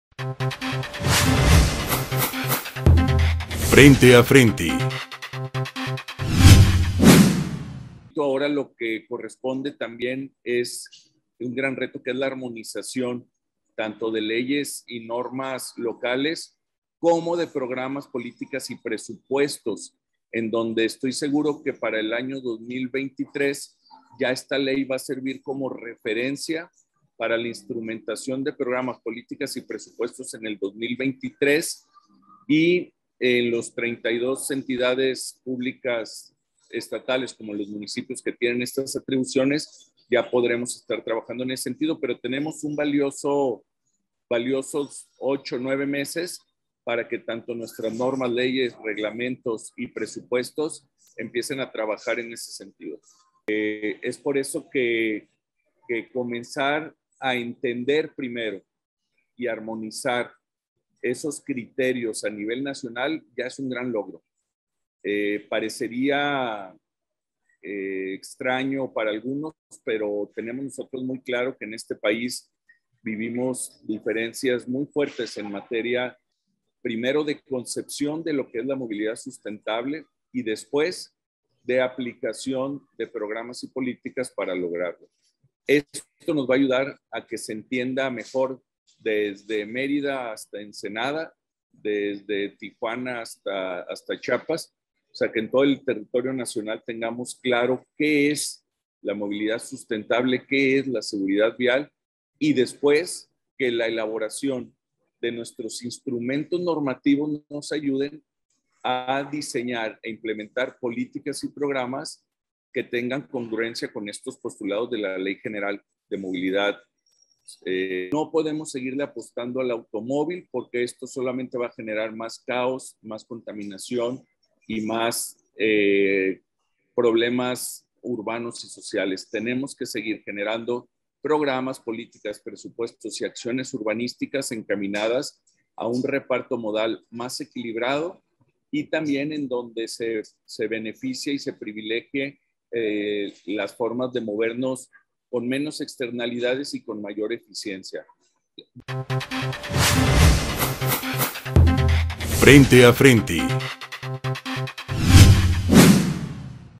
Entrevista a Diego Monraz, sobre LGMySV
Diego Monraz habla del reto de armonizar y crear leyes y reglamentos estatales que permitan unificar los criterios de la movilidad a nivel nacional, así como enfocar presupuestos a esta tarea.